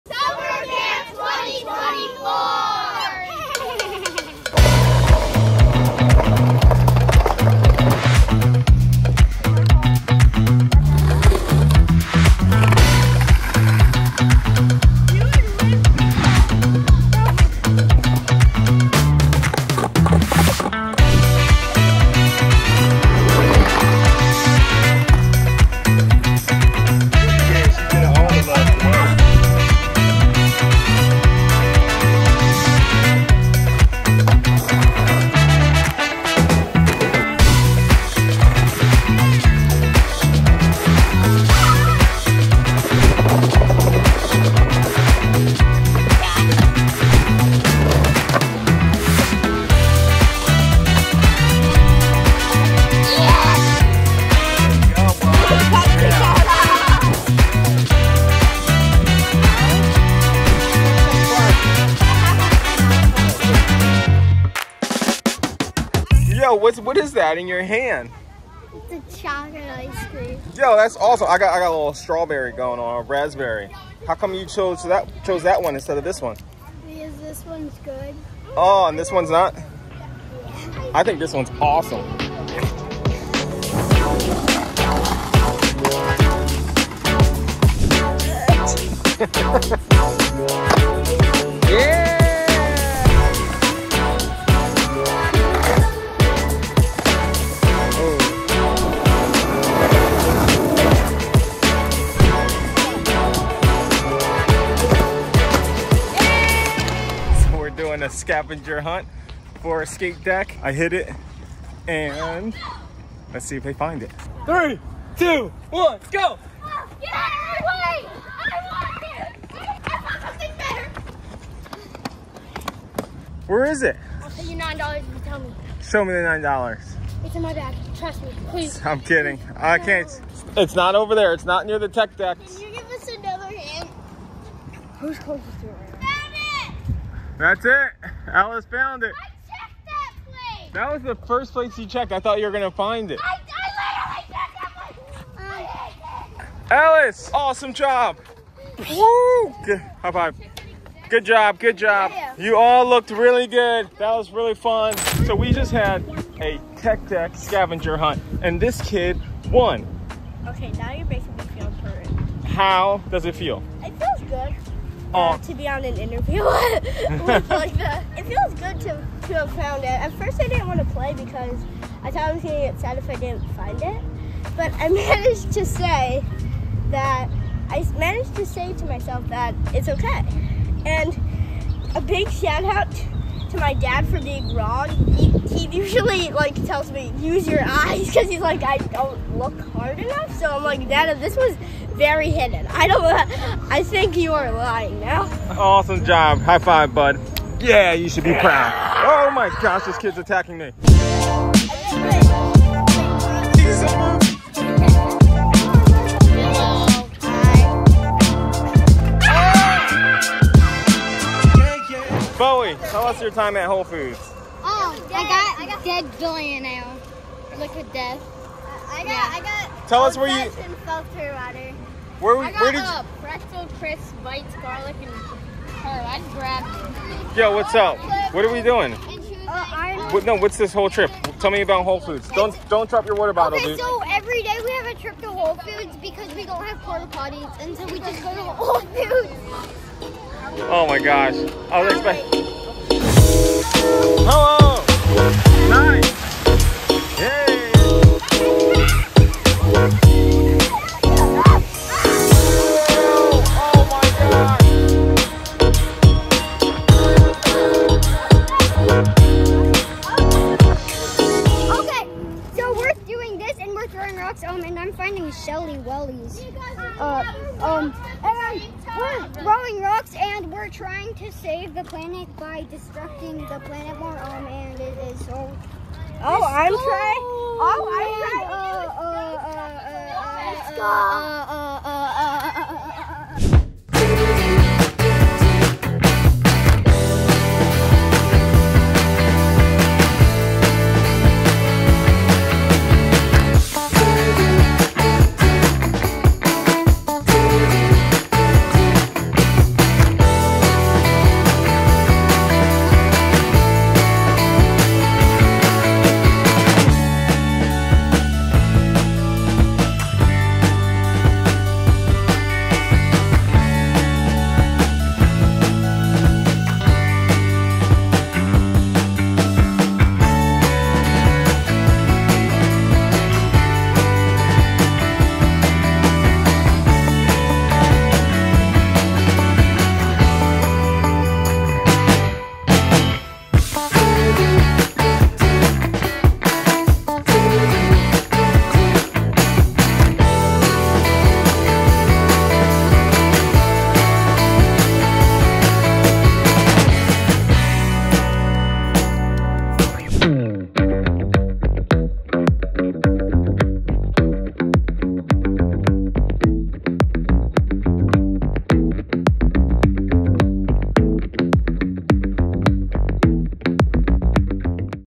Summer Camp 2024, Week 2: Skateboarding, Games, and Non-Stop Fun at Paine's Park!
Summer Camp 2024, Week 2 at Paine's Park! The heat couldn’t stop the fun! We had an absolute blast from start to finish. Our days were packed with skateboarding, thrilling games, and delicious lunch breaks at Whole Foods. We also went on exciting local field trips, adding even more adventure to our week. It was non-stop excitement and laughter all the way!

We can’t wait to see everyone at the next camp!